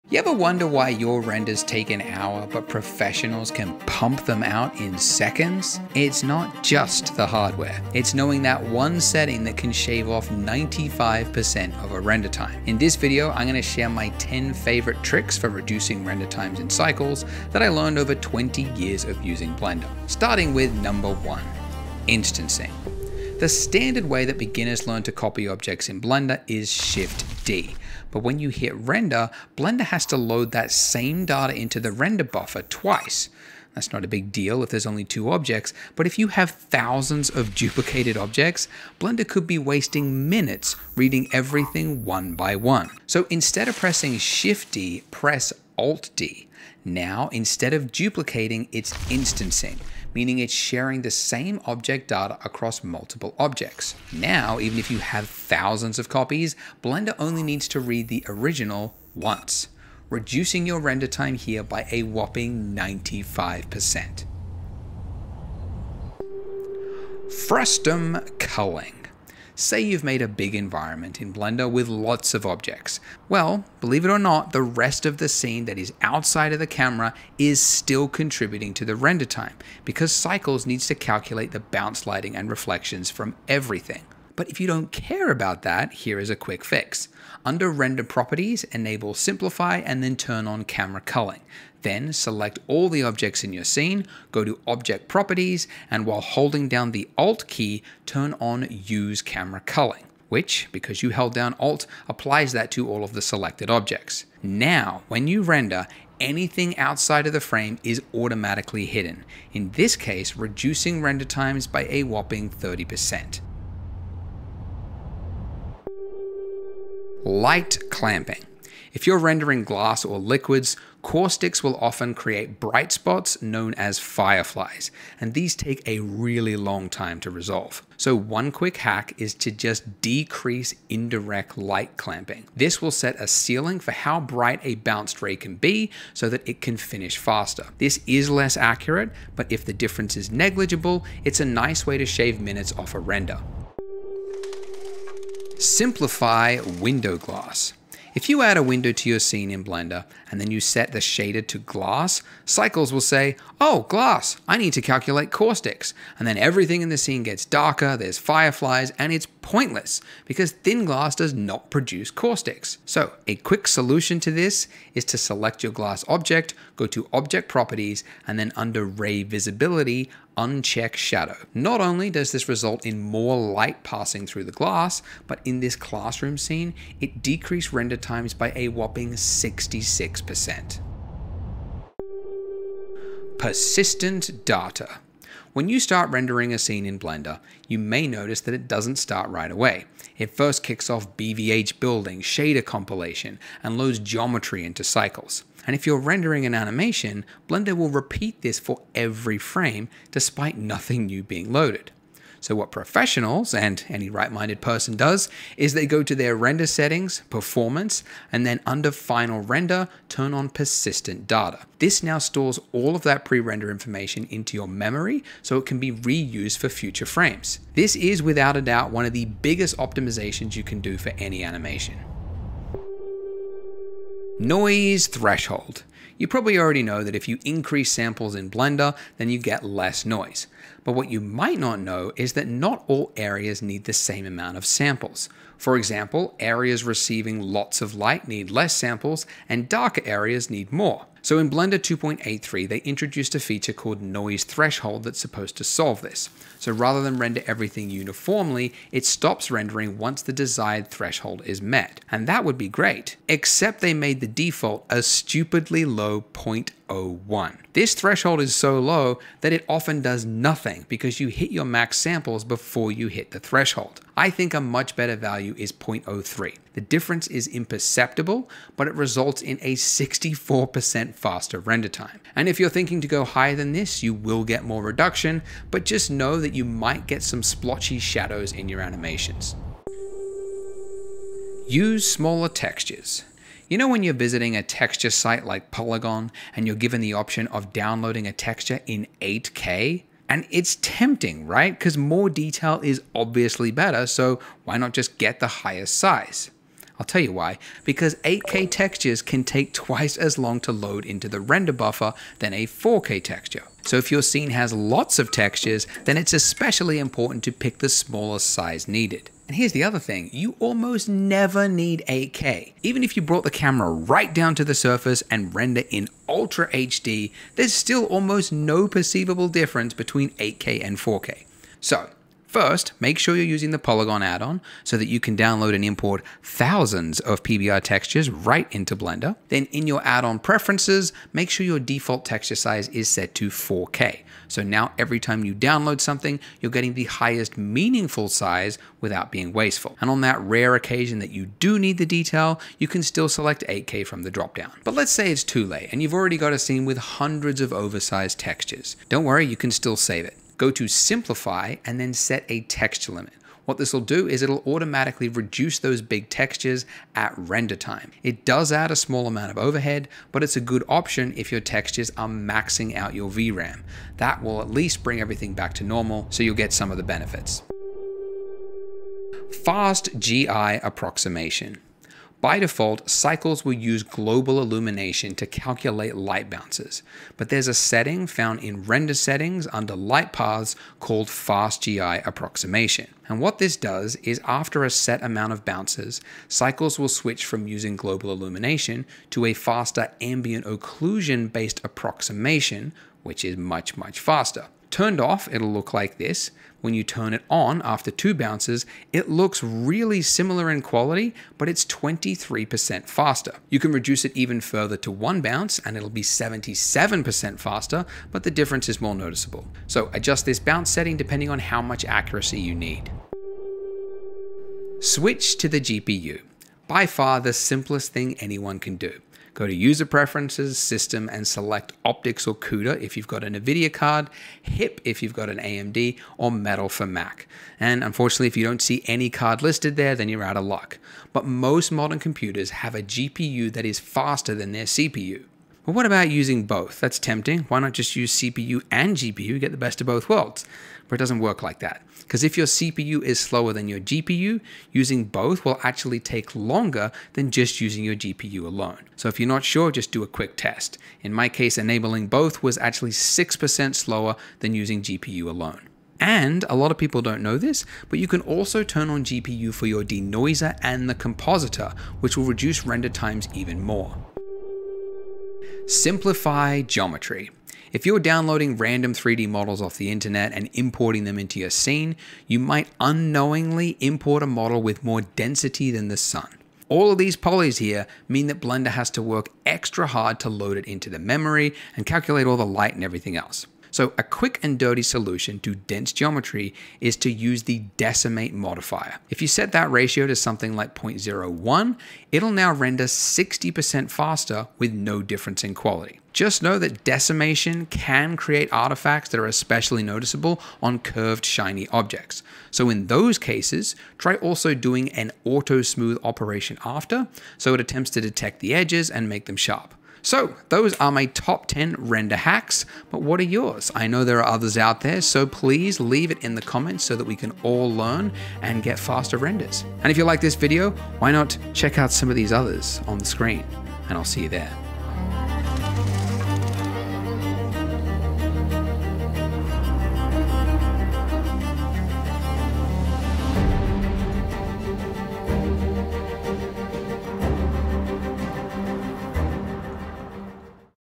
10 Easy Ways to Render Faster in Blender
10 render hacks I learned after 20 years of using Blender.

=== Chapter Marks ✂️===
0:00 Intro
0:19 Instancing
1:05 Frustum Culling
1:55 Light Clamping
2:23 Simplify Window Glass
3:01 Persistent Data
3:42 Noise Threshold
4:39 Use Smaller Textures
6:07 Fast GI Approximation
6:53 Switch to GPU
7:44 Simplify Geometry

===Follow me🫰===